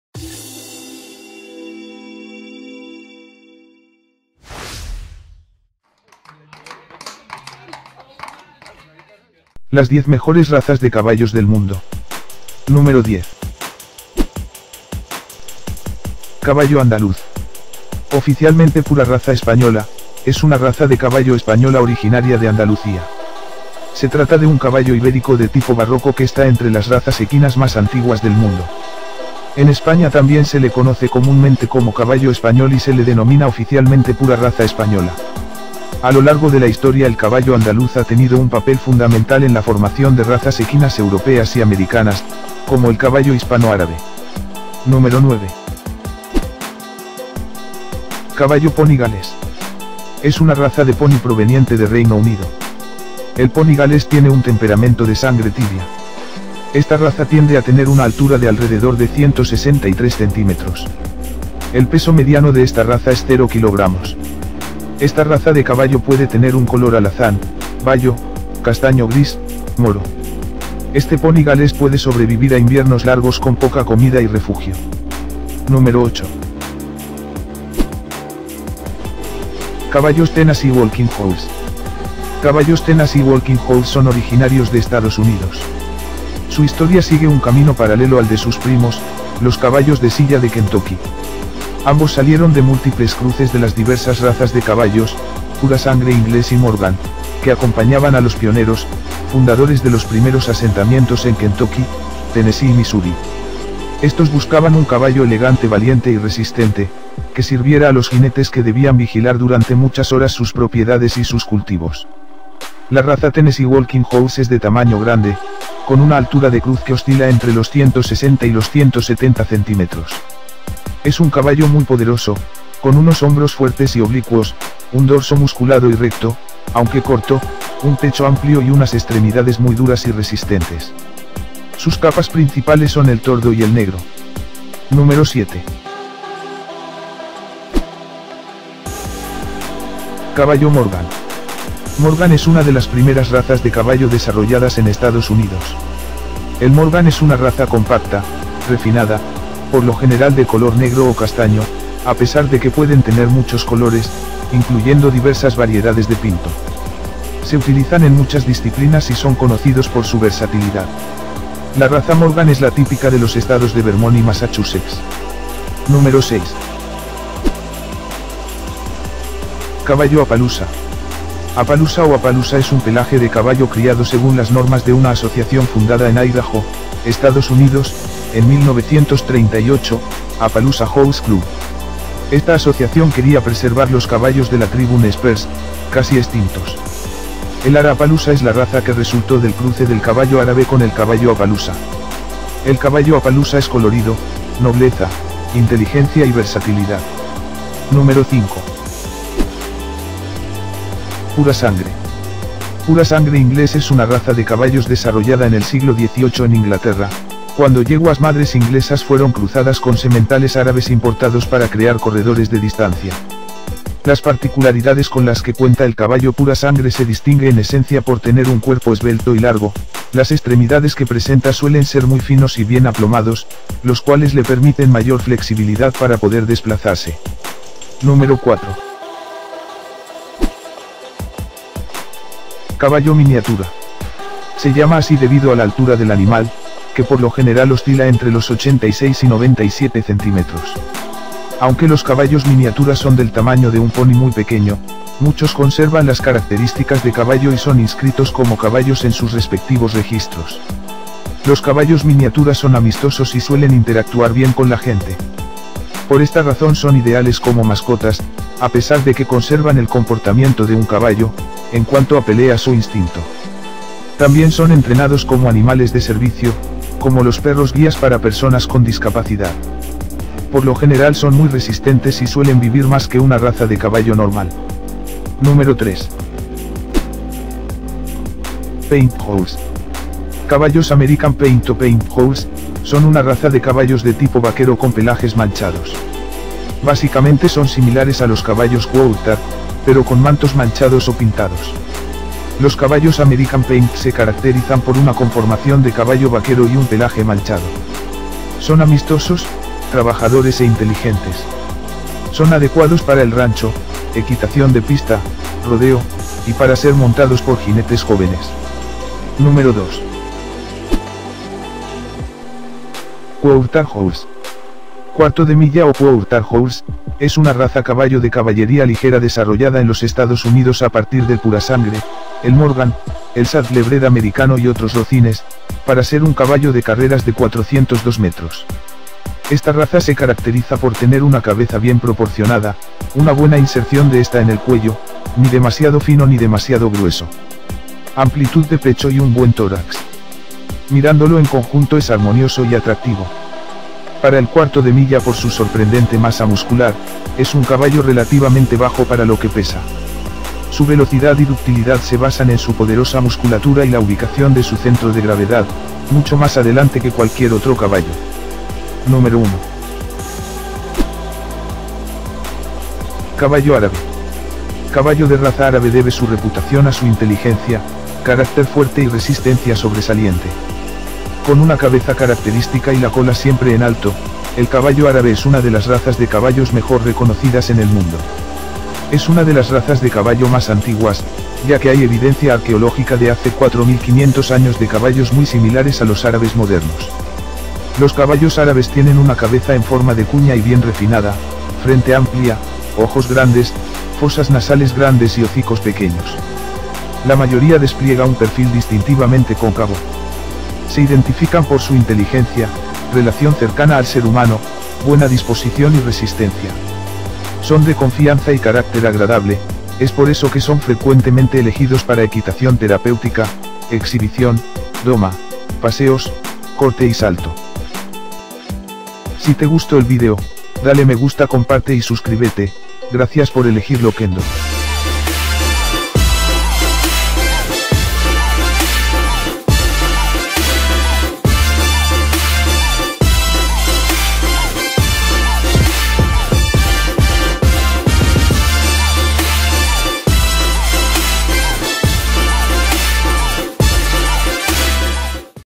Las 10 Mejores Razas De Caballos Del Mundo
Bienvenidos a Top Lokendo.  Espectaculares ejemplares pura sangre, caballos exóticos. Estas son Las 10 Mejores Razas De Caballos Del Mundo. @TopLokendo